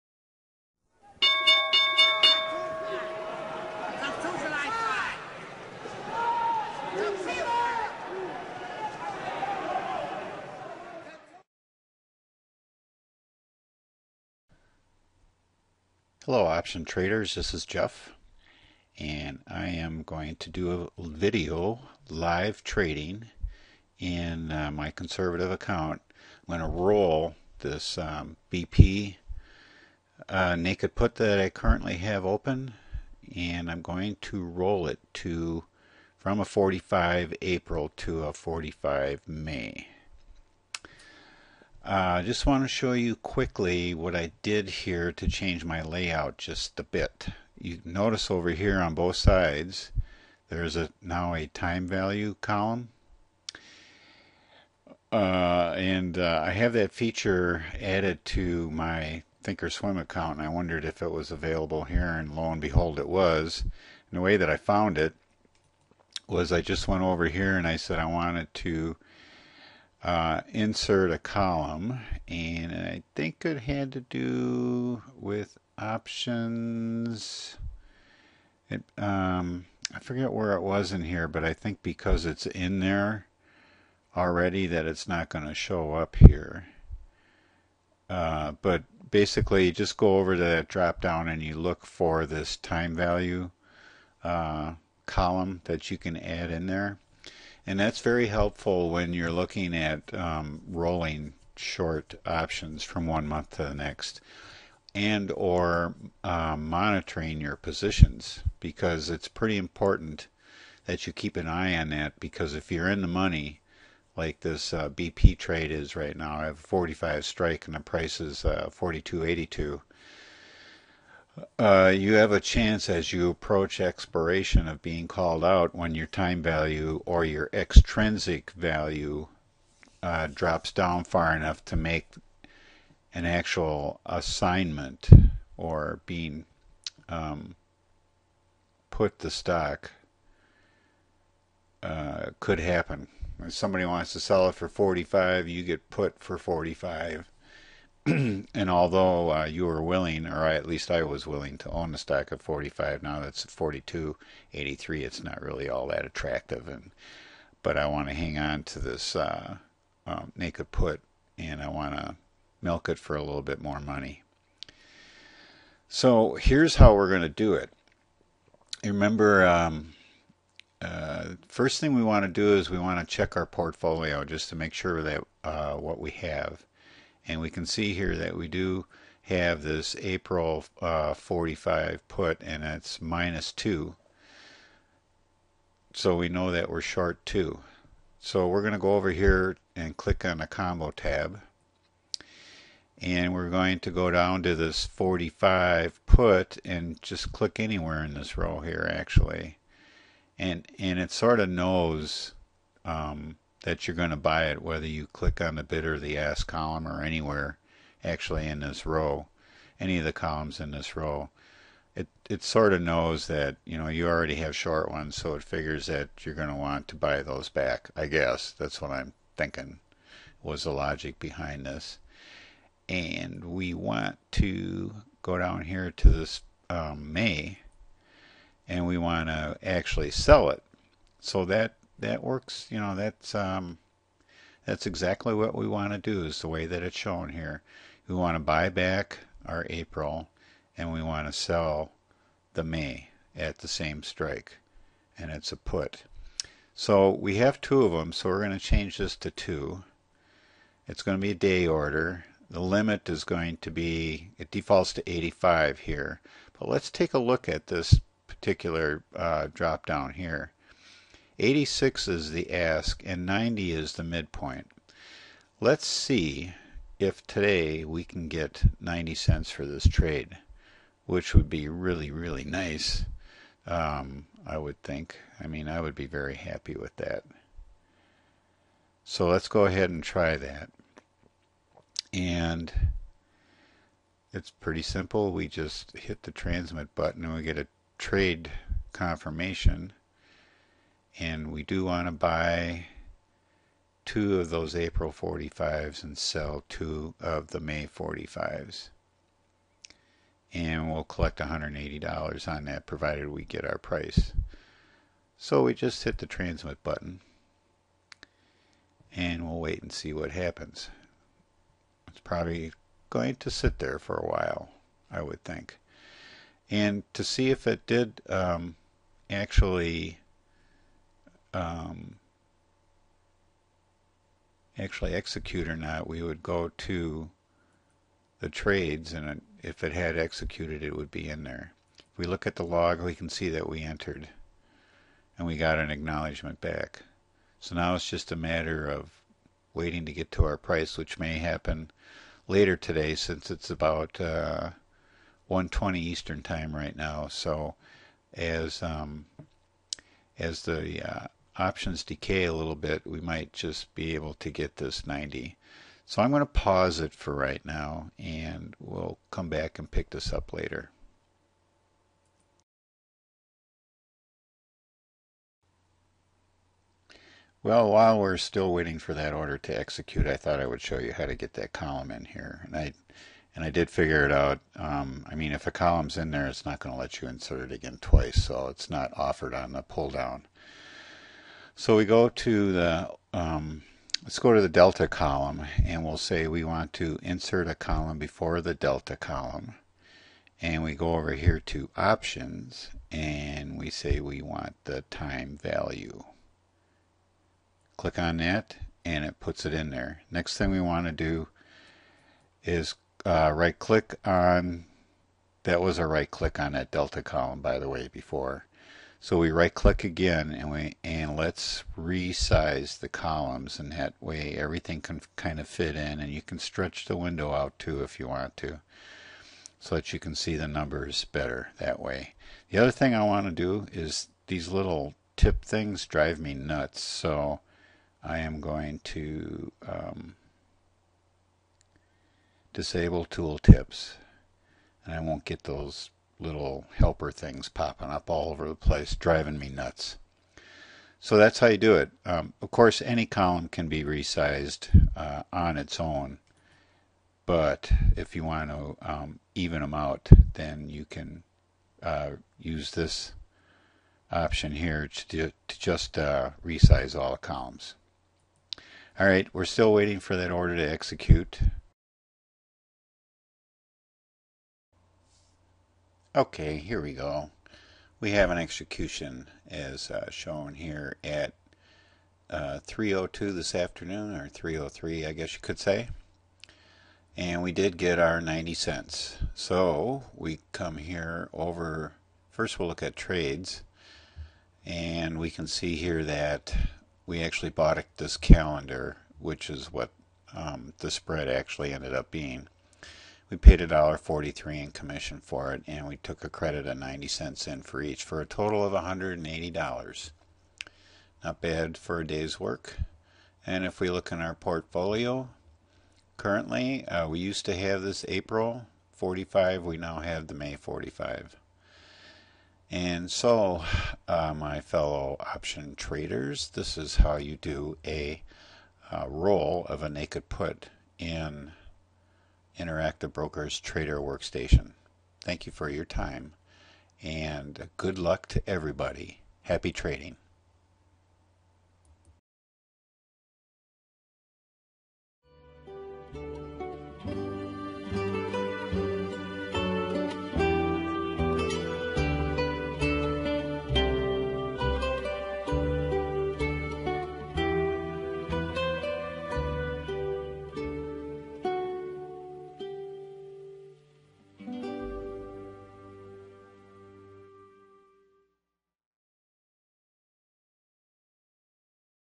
Rolling a Naked Put in TWS
This video shows one way to roll a Naked Put from one month to the next at the same strike using Interactive Brokers Trader Workstation (TWS).